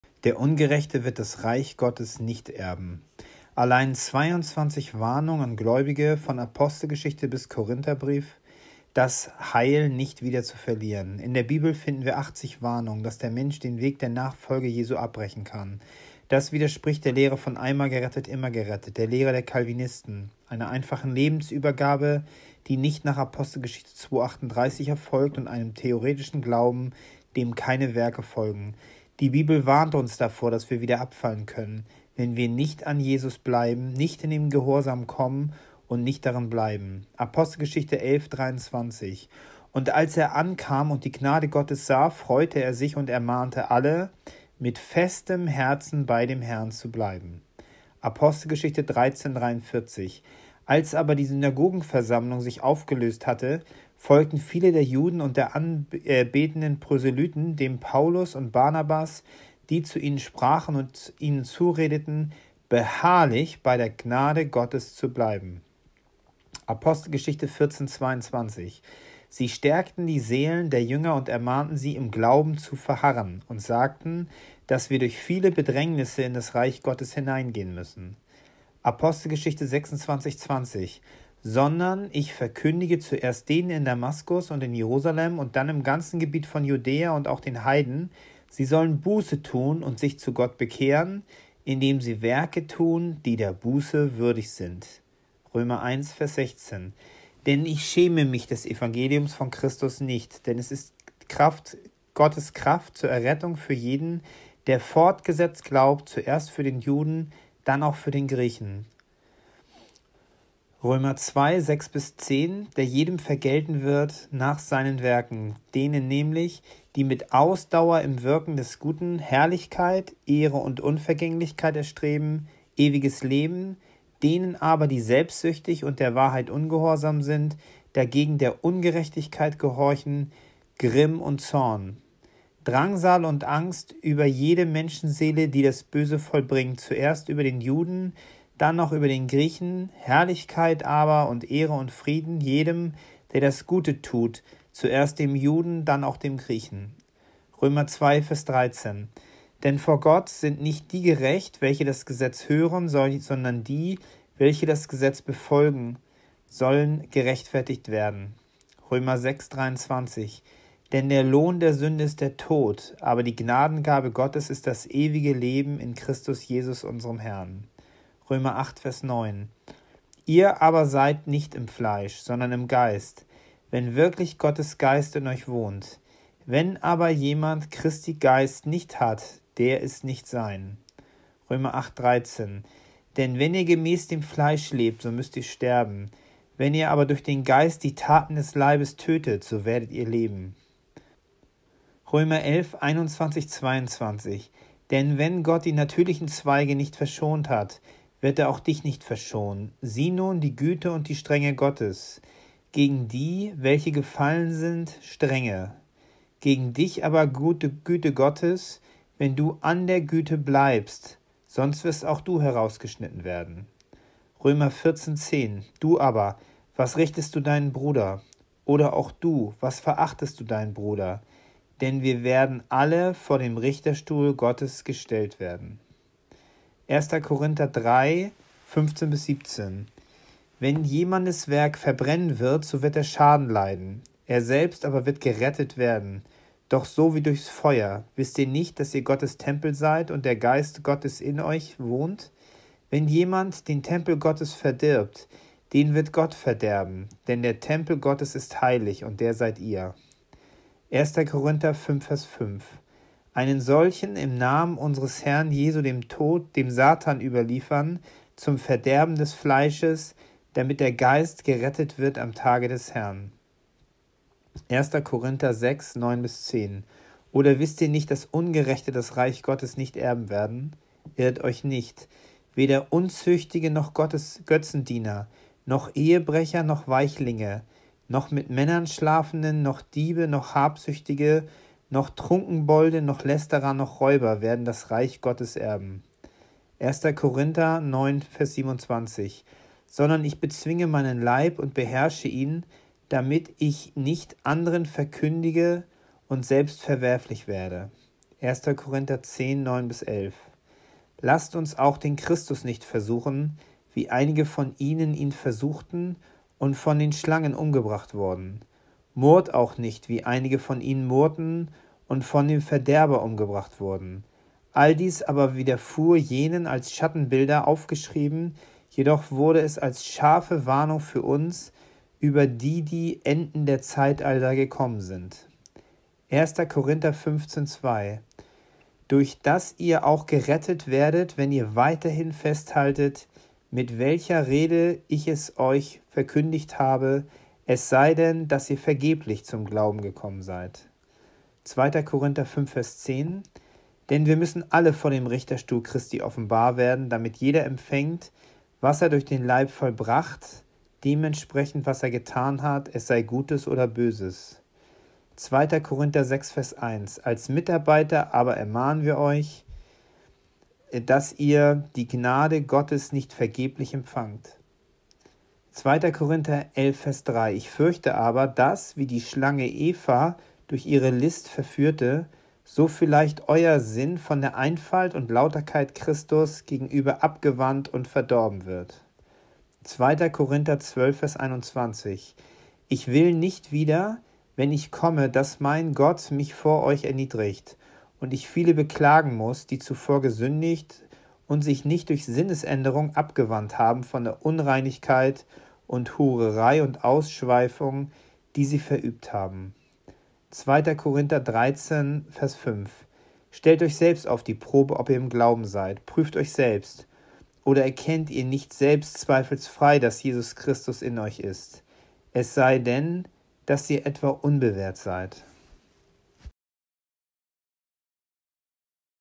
Der Ungerechte wird das Reich Gottes nicht erben - 22 Warnungen an Gläubige von APG bis 2. Kor.
Allein 22 Warnungen an Gläubige von Apostelgeschichte bis Korintherbriefe, das Heil nicht wieder zu verlieren!

In der Bibel finden wir 80 Warnungen, dass der Mensch den Weg der Nachfolge Jesu abbrechen kann. Das widerspricht der Lehre von „einmal gerettet – immer gerettet“, der Lehre der Calvinisten, einer einfachen Lebensübergabe, die nicht nach Apostelgeschichte 2,38 erfolgt und einem theoretischen Glauben, dem keine Werke folgen. Die Bibel warnt uns davor, dass wir wieder abfallen können, wenn wir nicht an Jesus bleiben, nicht in den Gehorsam kommen und nicht darin bleiben: